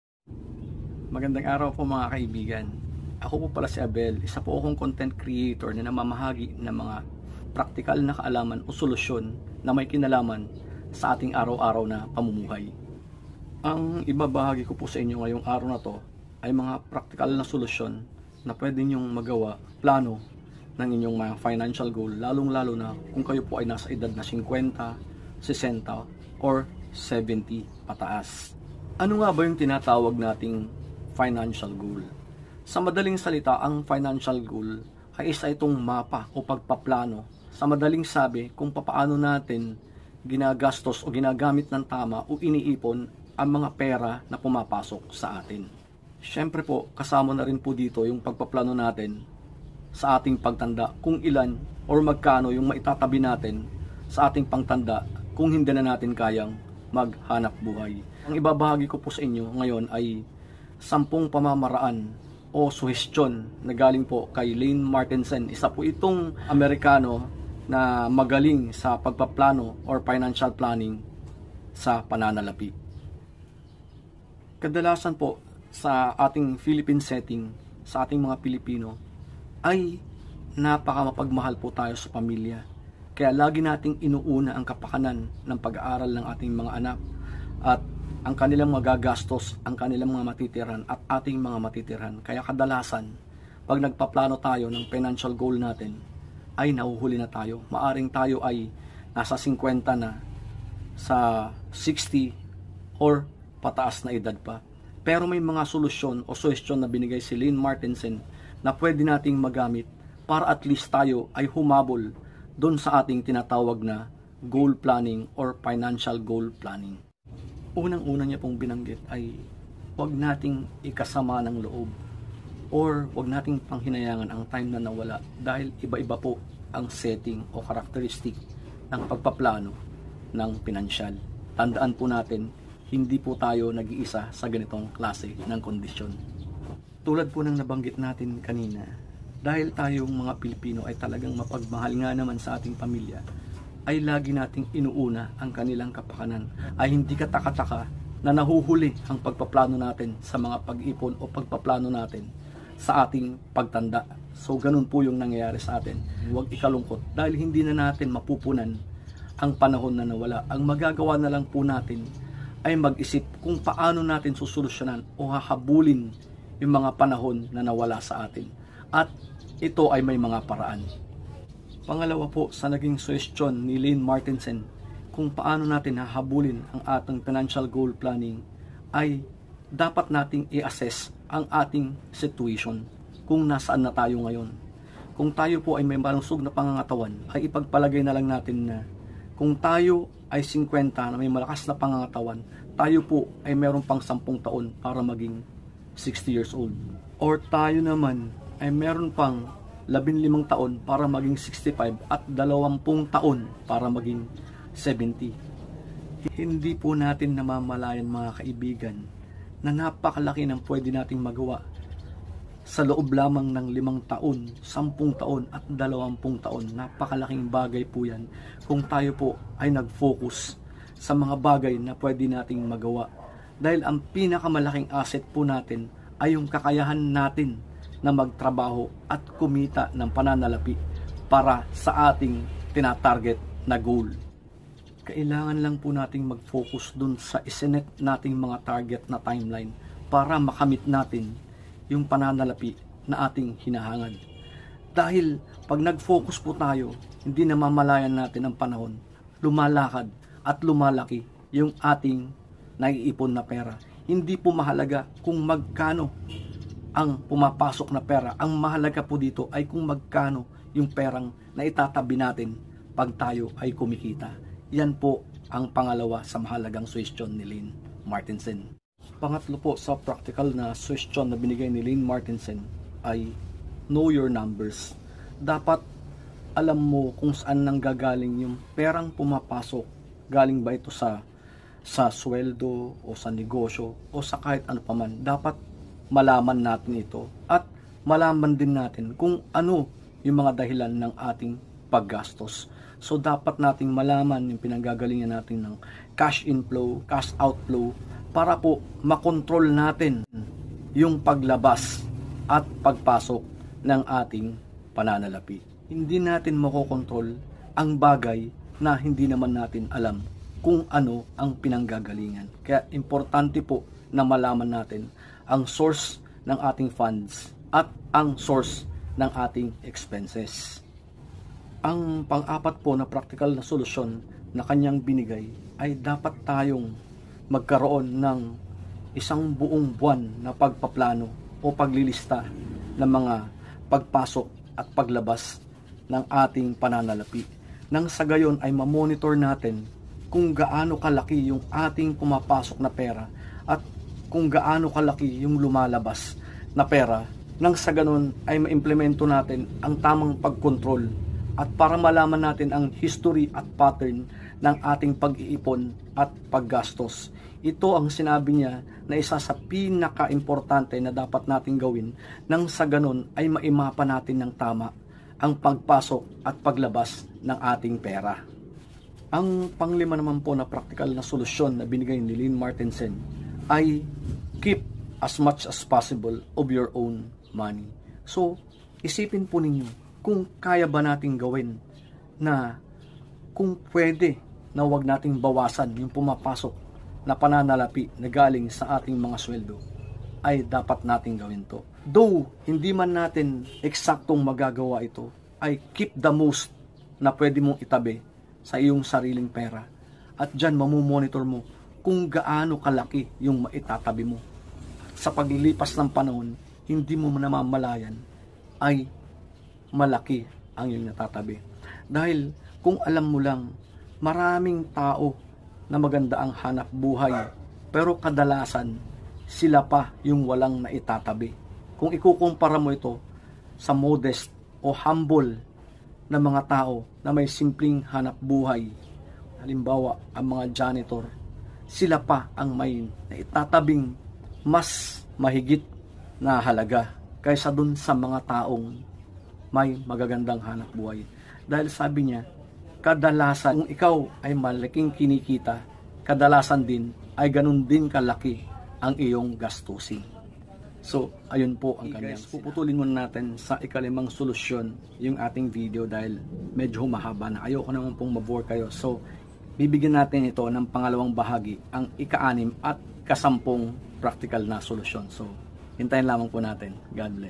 Ages 50s 60s 70s Financial goal strategy that works Part 1
This video is about the strategy that you can do if you have little or no enough financial savings available for you during your retirement age.

I have put into filipino language the strategies discuss by Lane Martinsen a known financial strategies.

i have discussed first five of his strategies for the first part of the video and il be uploaxing the second part as soon as its available.